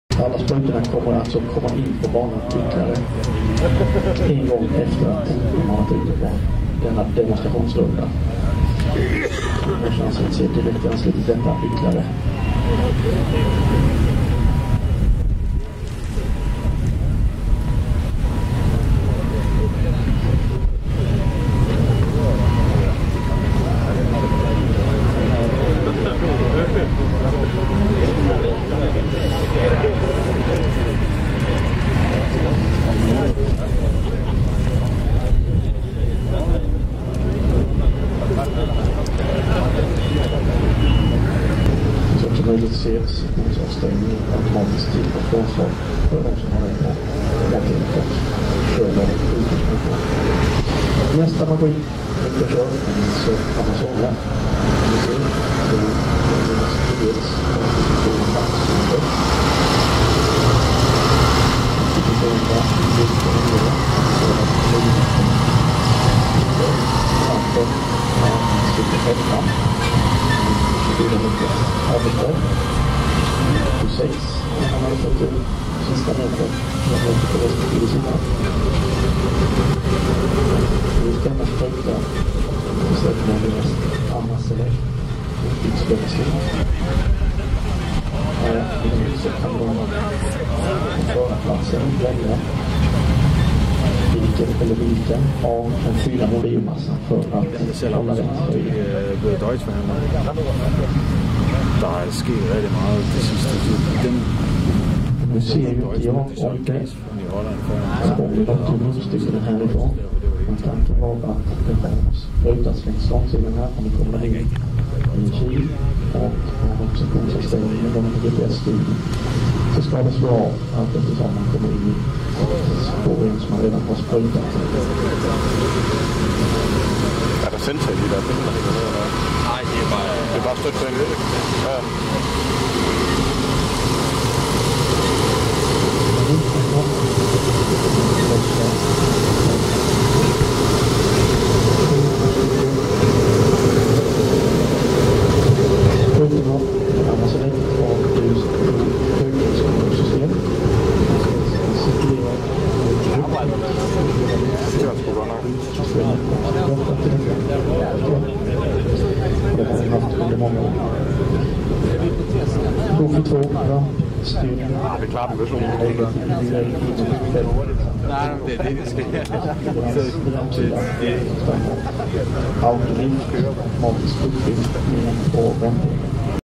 Amazone UX 5200 Super Sprayer Borgeby Agricultural Fair 2017
Amazone ux 5200 GPS shown at agricultural fair for professional farmers sprayer demo